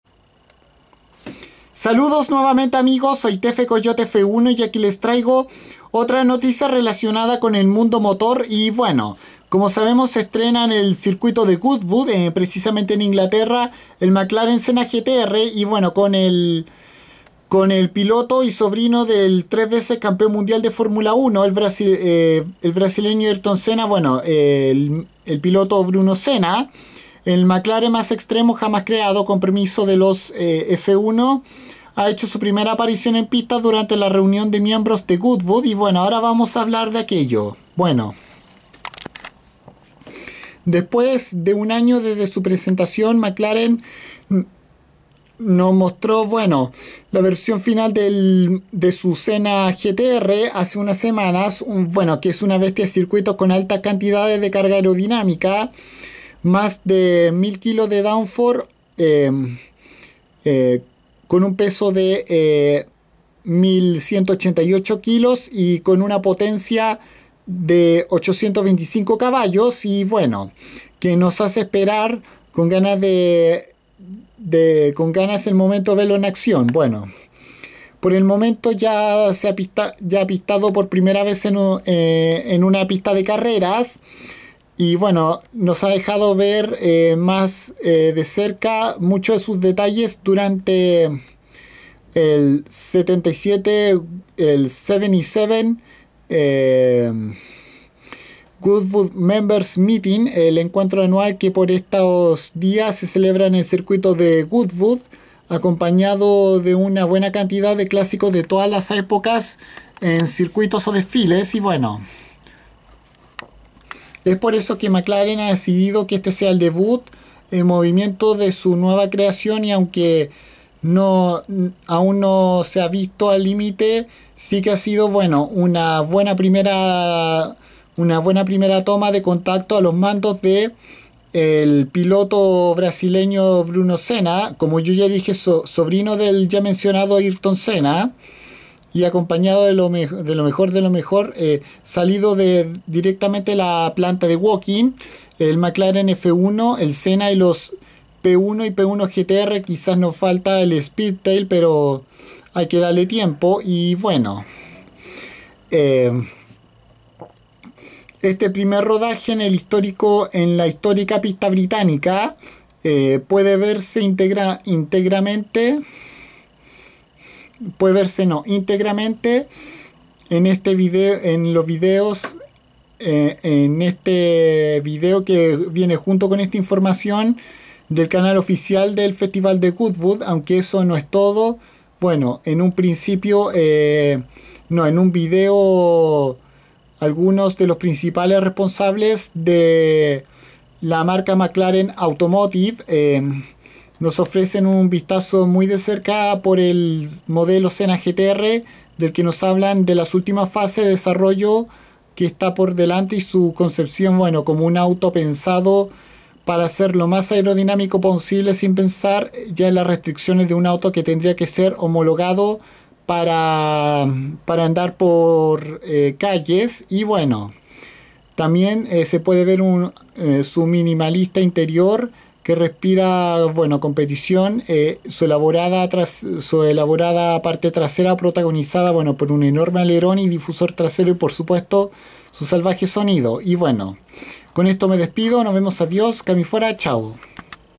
El Mclaren Senna GTR debuta en el Circuito de Goodwood
De la mano del piloto brasilero Bruno Senna sobrino del tricampeón de Fórmula 1 Ayrton Senna este modelo se estrena en la mítica pista británica de Goodwood